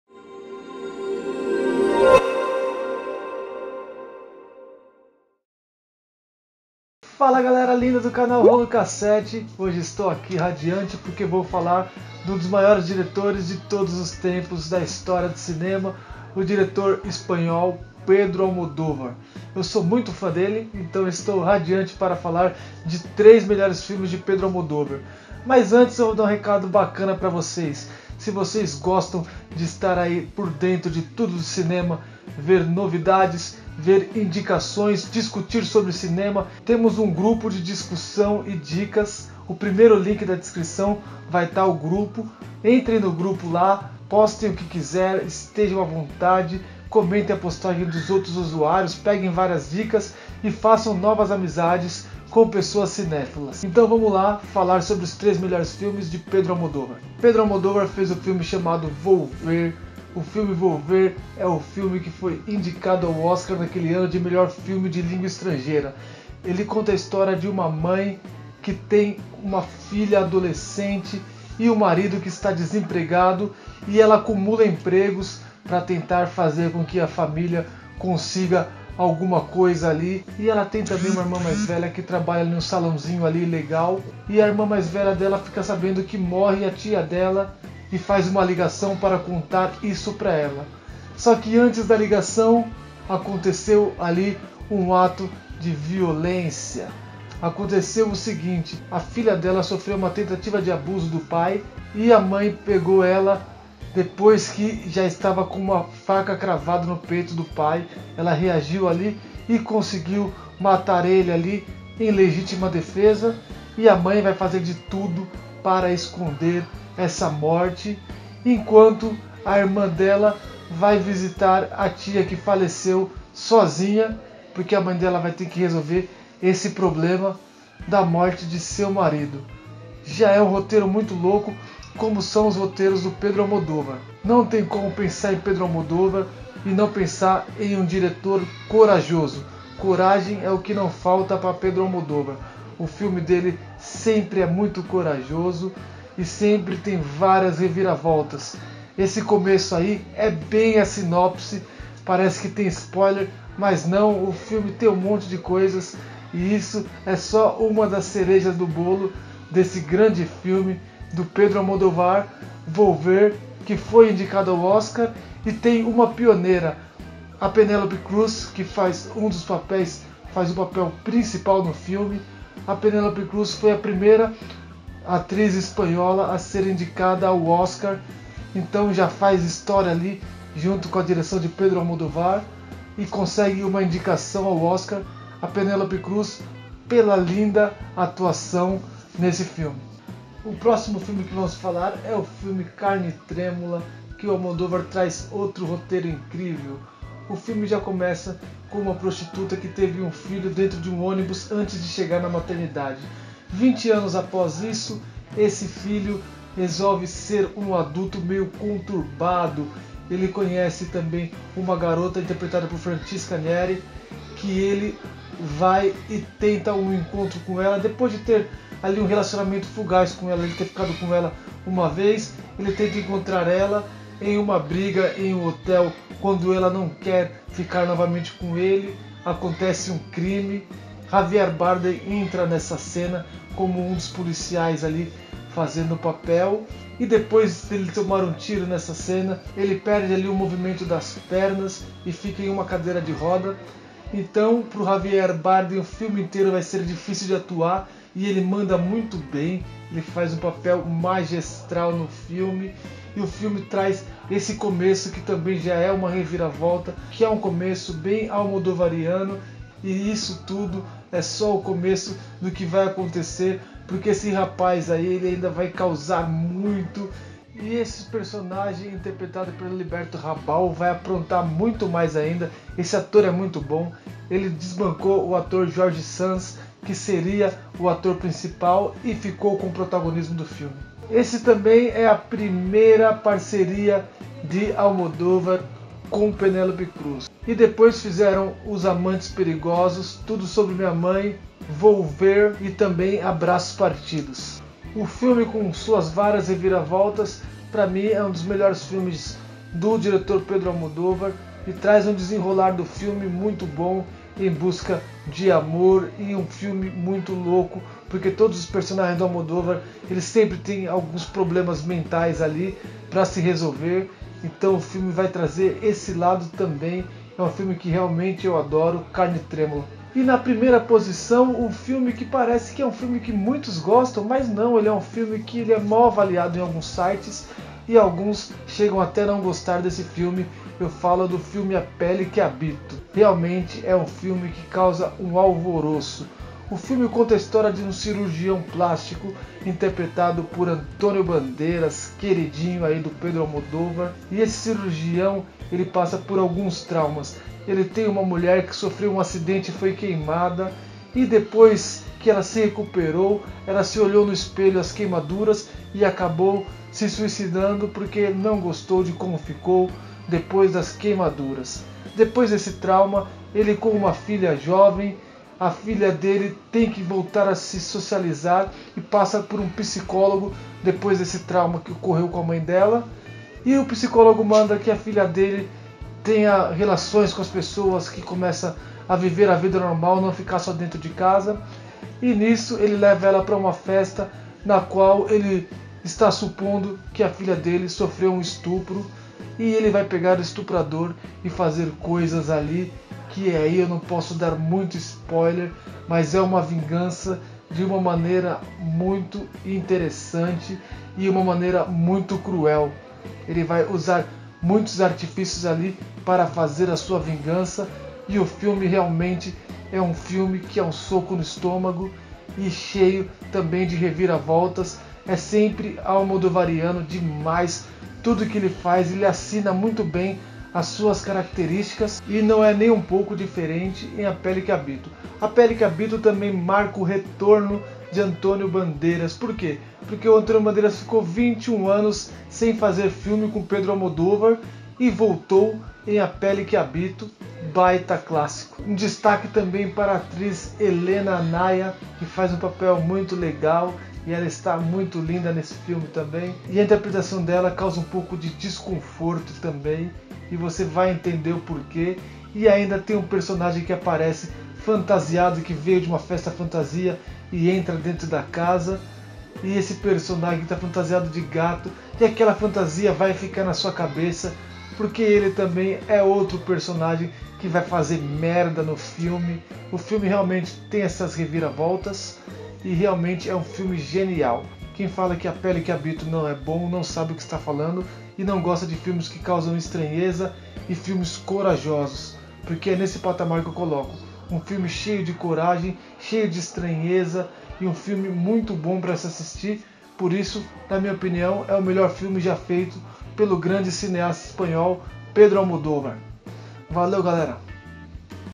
3 filmes de Pedro Almodóvar + curiosidades
3 filmes de Pedro Almodóvar + curiosidades
Neste vídeo você conhecerá 3 excelentes filmes do grande diretor espanhol Pedro Almodóvar e ainda muitas curiosidades, filmes como A pele que habito, Carne trêmula e Volver.

TWEETER: @k7rolo